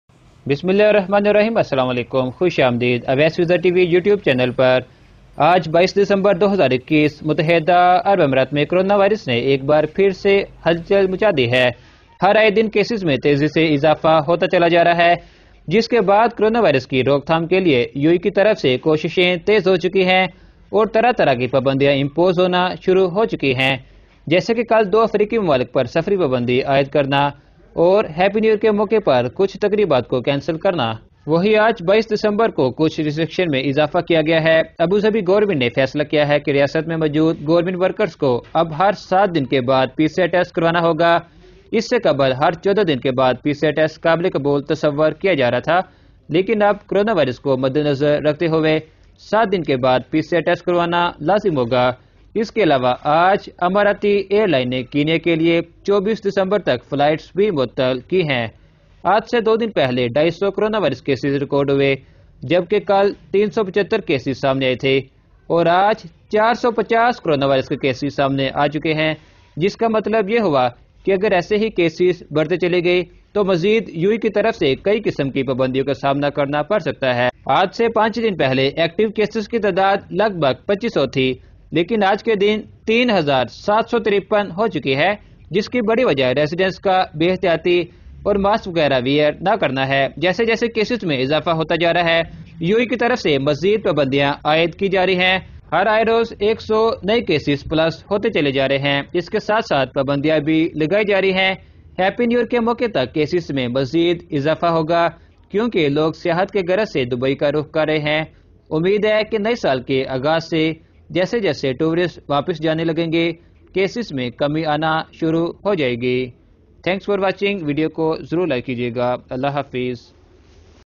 UAE Dubai Today New Restrictions:Coronavirus Virus Cases Rises Again
Uae coronavirus report today
Dubai Coronavirus Cases Report Today Update
Dubai Travel new rule today news
Pakistan india flights update